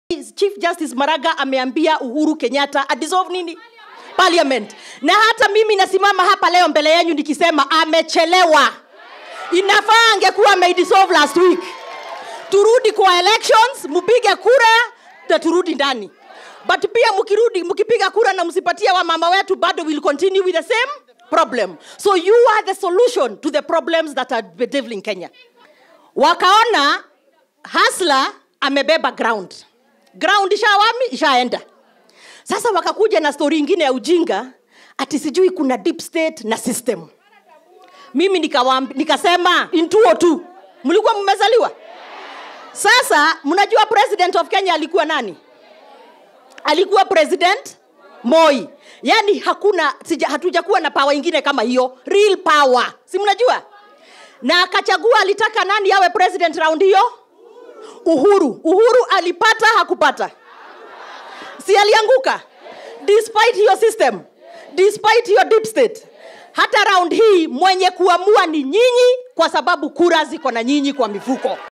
Senator Susan Kihika
We are making bold commitment to supporting women in starting and running their own businesses with the utmost appreciation that no instrument for the transformation of our country is as powerful as the empowerment of women.